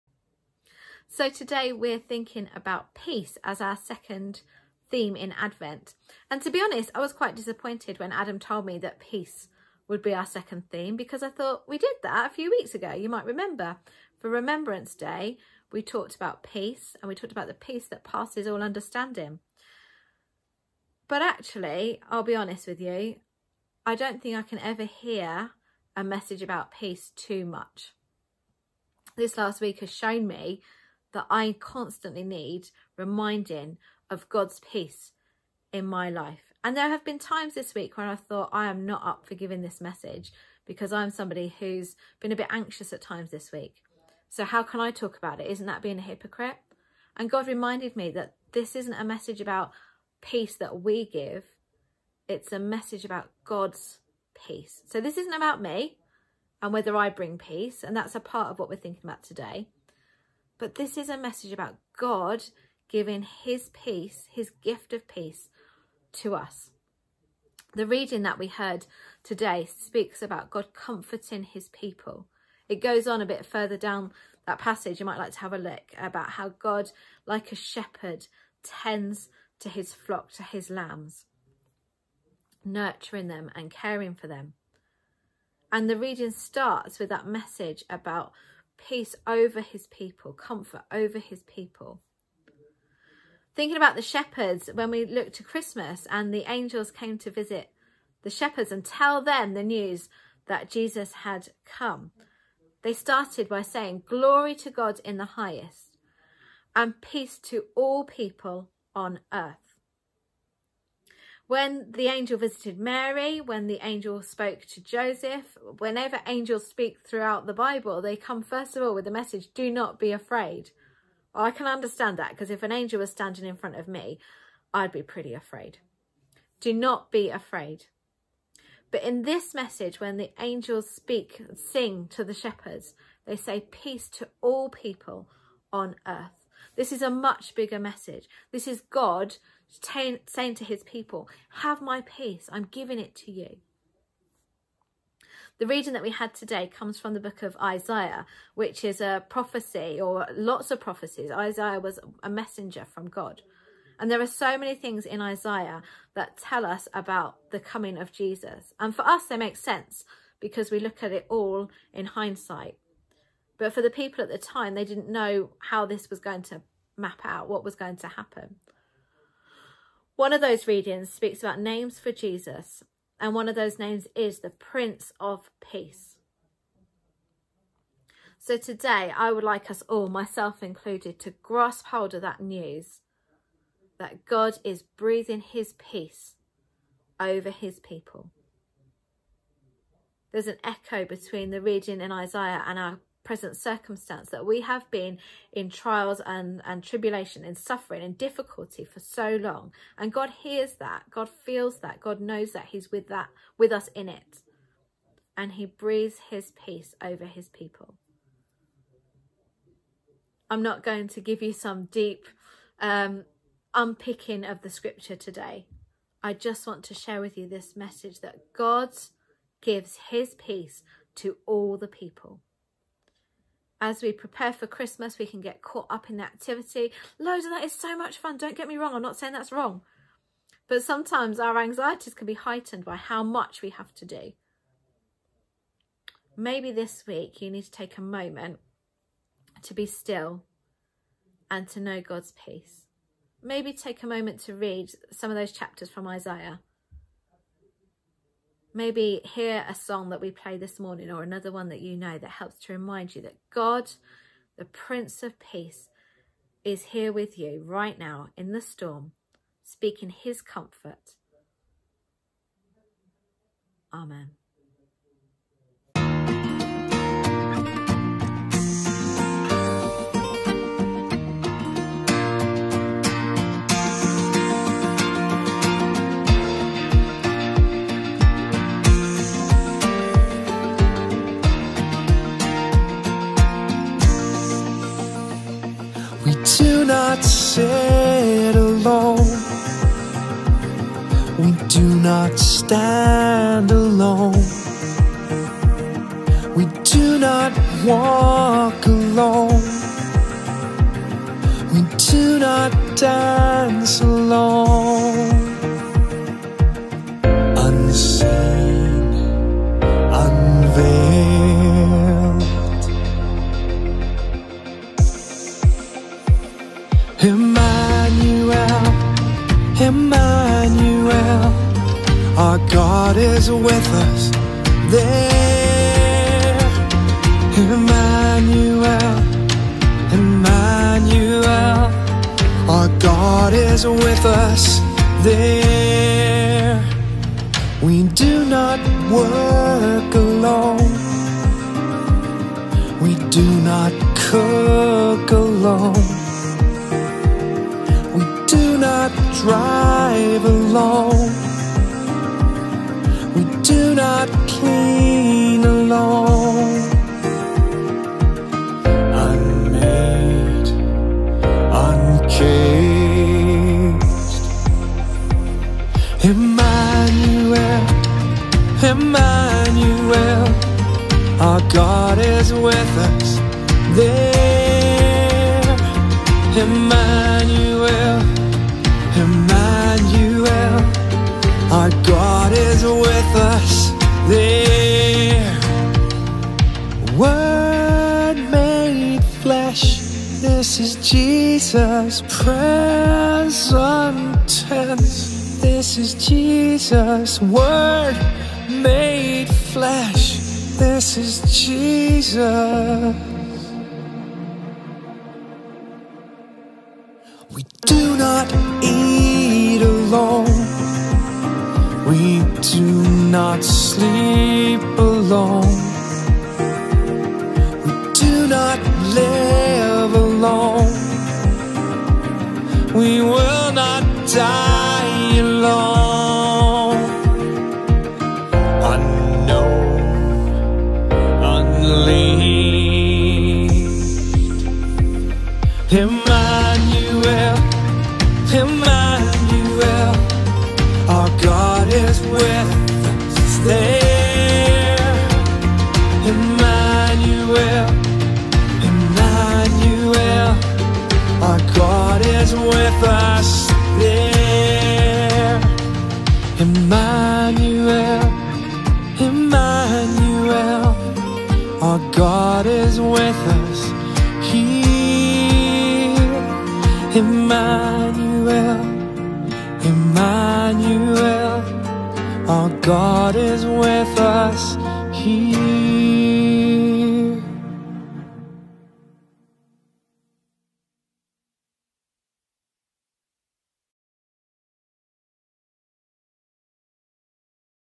Advent Peace - Message replay
This morning, at Breakfast Church, we had an excellent message on Advent Peace from Becky May, one of our leaders.

During Advent, each Sunday at 8.30pm, we are going to replay the message from our Breakfast Church just for adults to sit, watch and ponder.